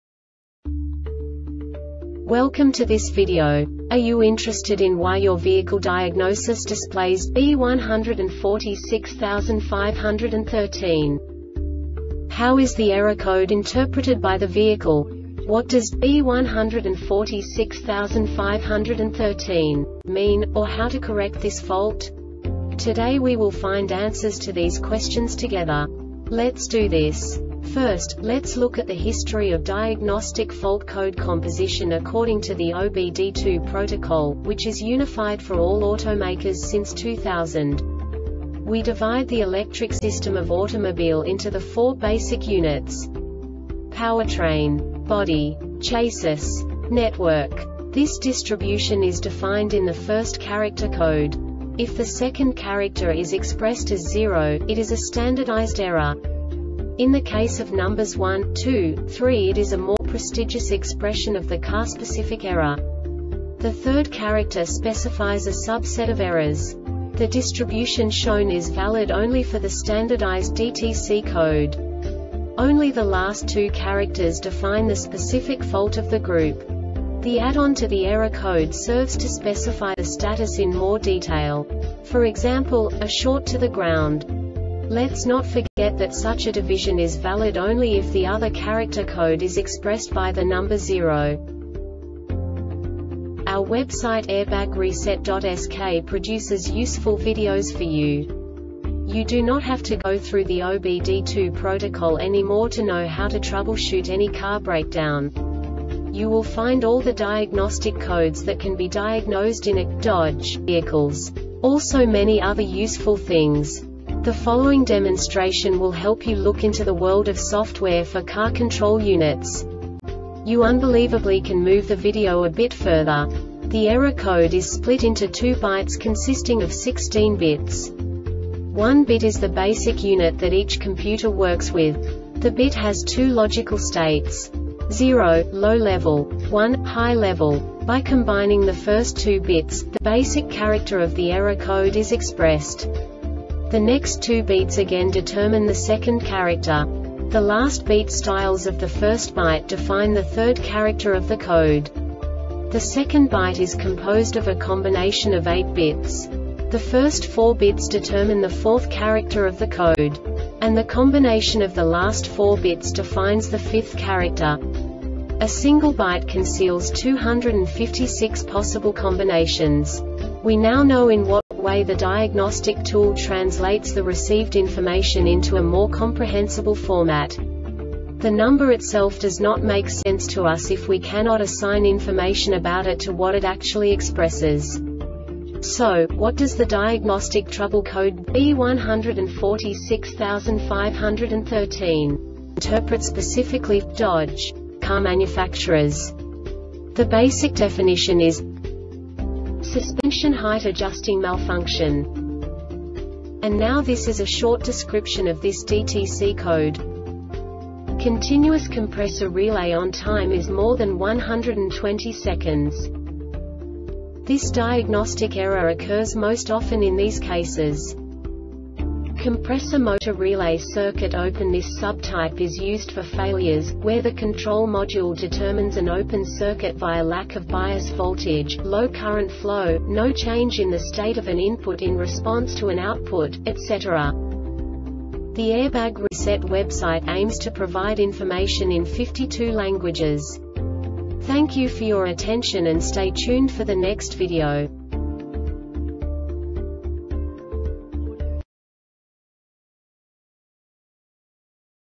DTC Dodge B1465-13 Short Explanation
Contents:
0:21 Basic DTC analysis according to OBD2 protocol standard.
1:48 Insight into programming
2:58 A diagnostically understandable description of the Diagnostic Trouble Code
3:05 Basic error definition

Make:
Dodge
Code:
B1465 13
Definition:
Suspension Height Adjusting Malfunction
Description:
Continuous compressor relay ON time is more than 120 seconds.
Cause:
Compressor motor relay
Failure Type:
Circuit Open